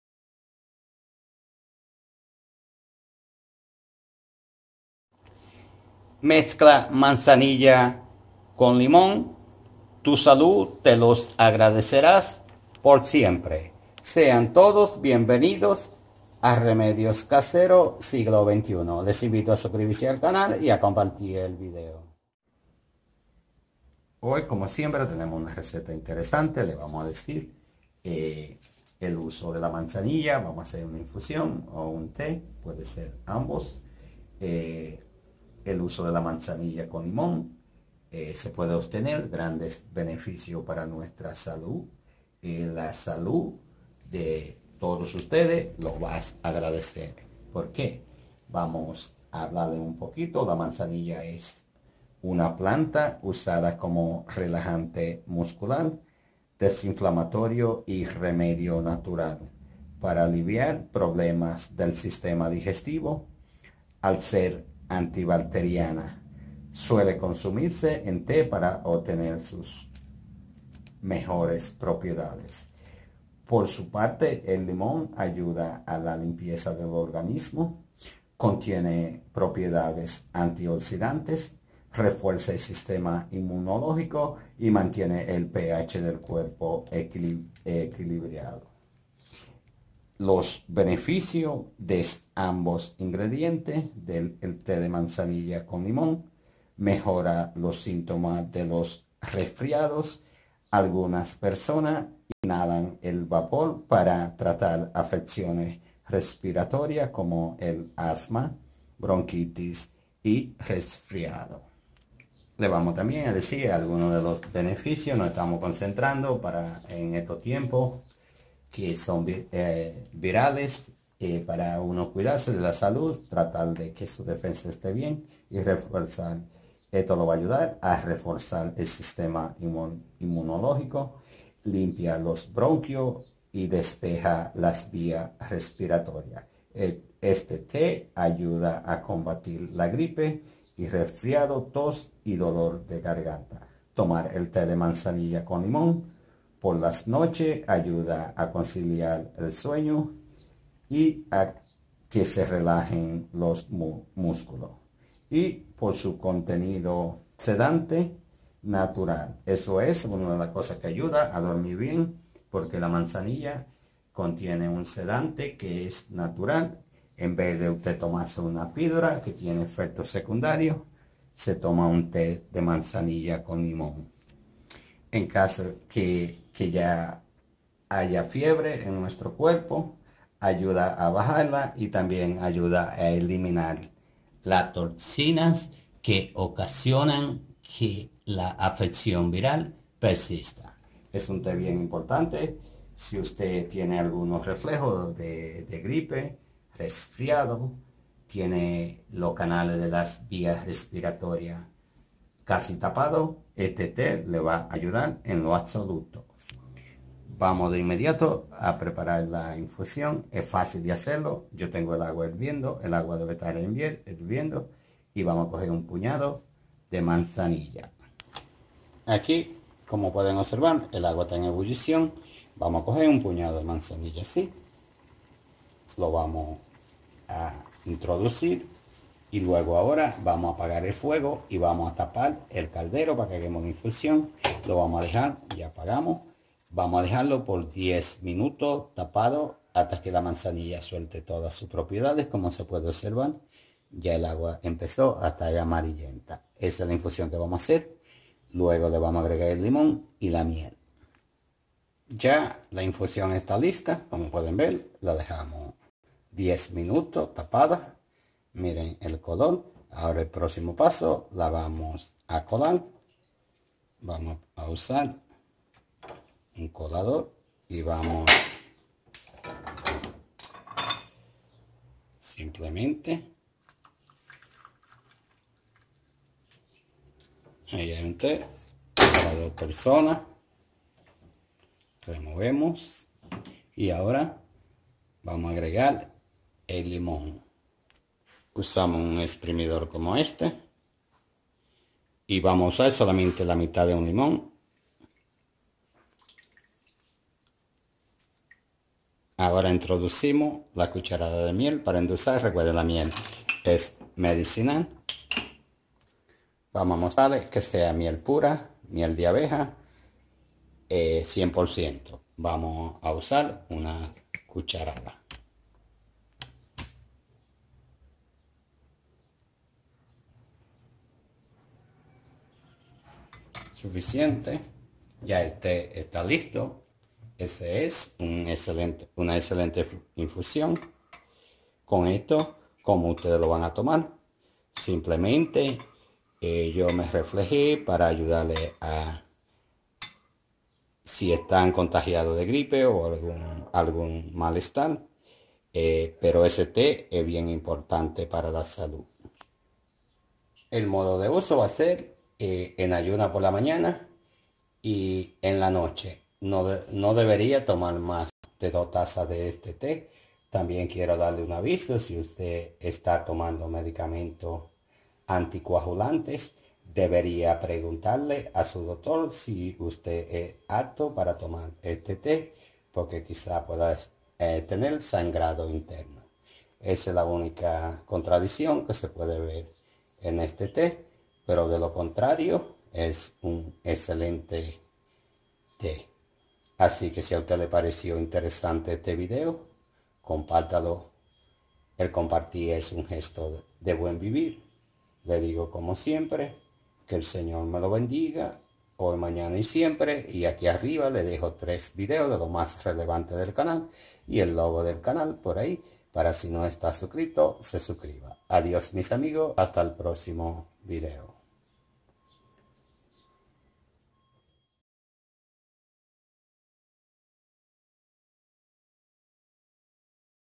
Mezcla Manzanilla Con Limón 🍋! Tu Salud te los Agradecerás Por Siempres
mezcla manzanilla con limón, tu salud te los agradecerás por siempres, Beneficios del té de manzanilla con limón, Poderoso antioxidante, La manzanilla es una planta utilizada como un relajante muscular,

desinflamatorio y remedio natural para aliviar problemas del sistema digestivo al ser antibacteriana. Suele consumirse en té para obtener sus propiedades.

Por su parte, el limón ayuda a la limpieza del organismo, contiene propiedades antioxidantes, refuerza el sistema inmunológico y mantiene el ph del cuerpo en equilibrio.
El té de manzanilla con limón mejora los síntomas de los resfriados. Algunas personas inhalan el vapor para tratar afecciones respiratorias como ser el asma, bronquitis y resfriados.

La forma más natural de preparar este té es con un manojo fresco de la planta y el jugo de un limón. Lo único que debes hacer es hervir un vaso de agua y colocar las flores al gusto con el jugo, luego necesitas cubrir el líquido y dejar en reposo alrededor de 10 a 15 minutos. Finalmente, disfruta de un delicioso té de manzanilla con limón caliente y relájate mientras aprovechas sus propiedades.

Precauciones De acuerdo con la Clínica Mayo, el uso de la manzanilla puede provocar reacciones alérgicas en algunas personas que son sensibles a la familia de plantas. Otros miembros de esta familia son la ambrosía, la caléndula, la margarita y el crisantemo. Sus efectos secundarios La manzanilla puede aumentar el riesgo de sangrado cuando se usa con medicamentos anticoagulantes. Así que antes de tomar esta infusión es recomendable saber la opinión de un especialista en caso de que puedas tener problemas.

La información que contiene todos los videos de este canal, es de carácter informativo.
Y por tanto no Pretende sustituir el consejo de un especialista médico.